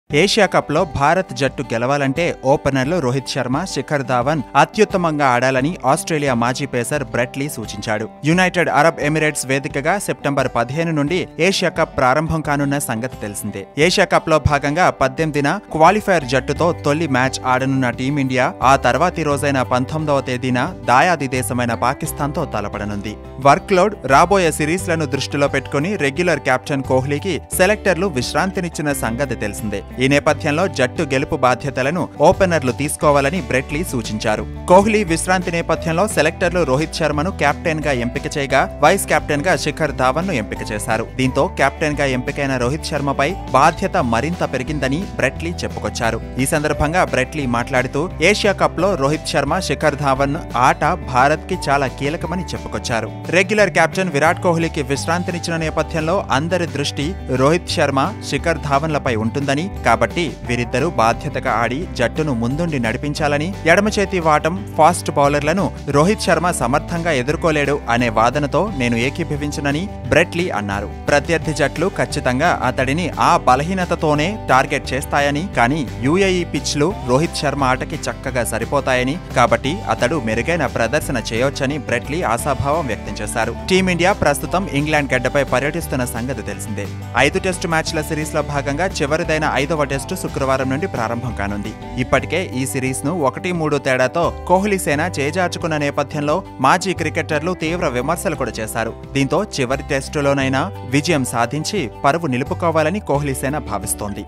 Rohit,Shikhar Key To India's Game-Plan In Asia Cup: Brett Lee | Oneindia Telugu
Former Australian cricket speedster Brett Lee has backed Rohit Sharma and Shikhar Dhawan to come good in the upcoming Asia Cup, saying the two hold the key to India's game-plan in the absence of regular skipper Virat Kohli.

ఆసియా కప్‌లో భారత్ జట్టు గెలవాలంటే ఓపెనర్లు రోహిత్ శర్మ, శిఖర్ ధావన్‌‌ అత్యుత్తమంగా ఆడాలని ఆస్ట్రేలియా మాజీ పేసర్ బ్రెట్‌లీ సూచించాడు. యునైటెడ్ అరబ్ ఎమిరేట్స్ వేదికగా సెప్టెంబరు 15 నుంచి ఆసియా కప్ ప్రారంభంకానున్న సంగతి తెలిసిందే.ఆసియా కప్‌లో భాగంగా 18న క్వాలిఫయర్ జట్టుతో తొలి మ్యాచ్ ఆడనున్న టీమిండియా ఆ తర్వాతి రోజైన 19న దాయాది దేశమైన పాకిస్థాన్‌తో తలపడనుంది. వర్క్‌లోడ్, రాబోయే సిరిస్‌లను దృష్టిలో పెట్టుకుని రెగ్యులర్ కెప్టెన్ కోహ్లీకి సెలక్టర్లు విశ్రాంతినిచ్చిన సంగతి తెలసిందే. ఈ నేపథ్యంలో జట్టు గెలుపు బాధ్యతలను ఓపెనర్లు తీసుకోవాలని బ్రెట్ లీ సూచించాడు.